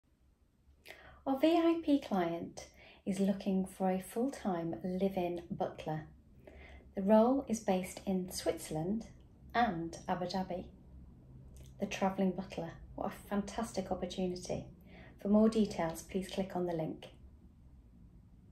Live-in Traveling Butler Job Offer Between Switzerland and Abu Dhabi, UAE I Morgan & Mallet Agency
Our VIP client is seeking an experienced live in-Houseman/Butler (M/F) who will oversee the day-to-day operations of their residences in Geneva, Switzerland and Abu Dhabi, UAE.
The candidate will be hands on, responsible for all the details of the home.
The position requires a high energy individual with an excellent work ethic and a sense of pride in work well done.
This is a permanent full-time position starting as soon as possible.